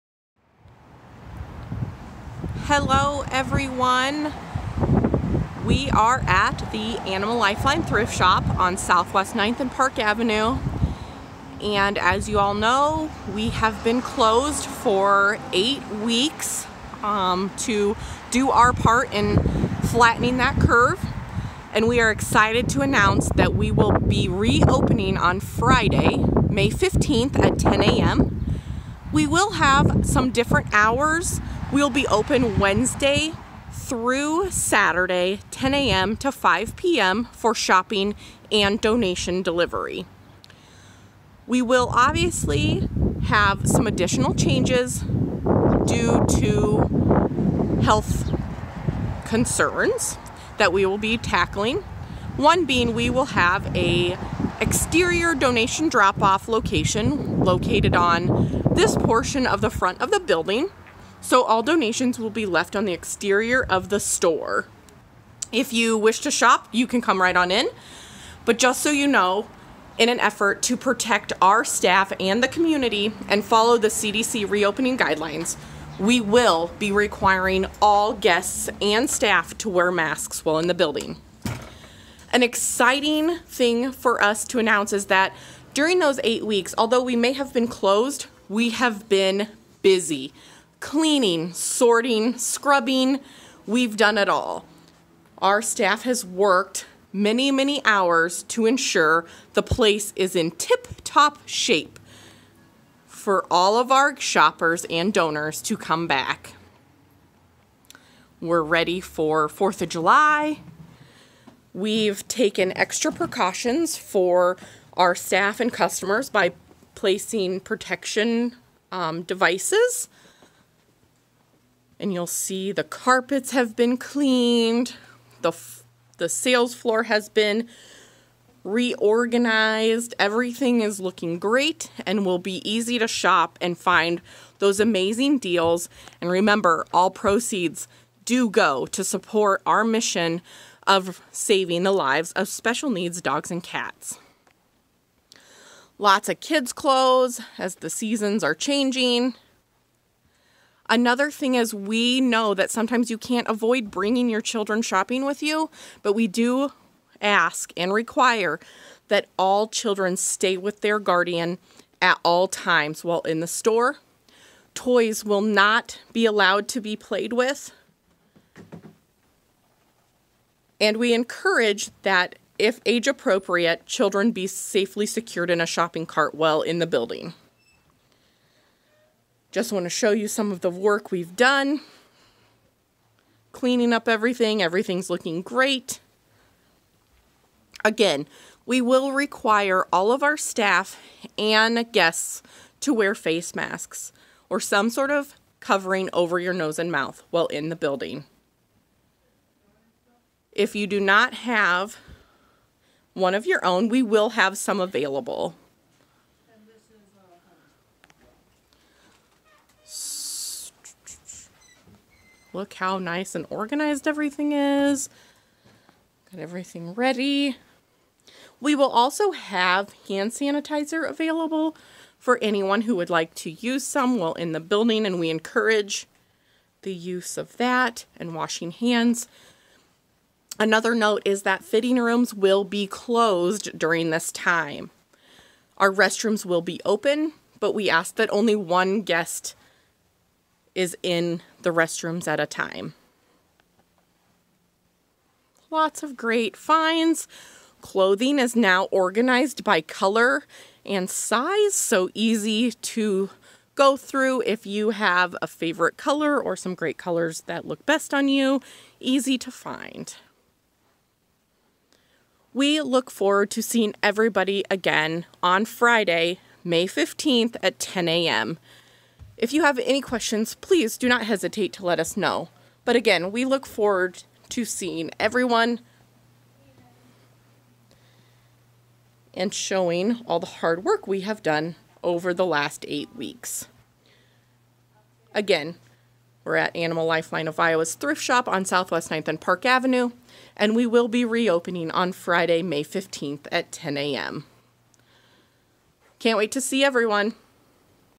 Thrift Shop Re-opening
Sneak peek of the Thrift Shop after closing due to Covid19 pandemic.